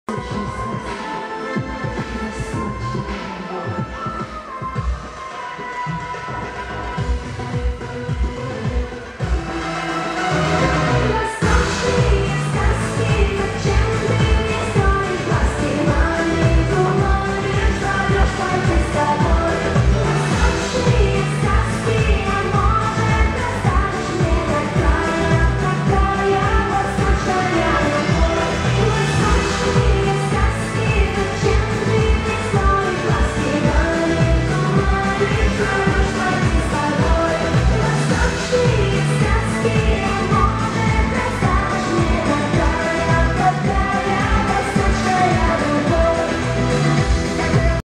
Amitié et Danse à Chatillon sur Loire juin 2012
Invitation de la troupe par l'école de danse "Les Sylphides" de Chatillon sur Loire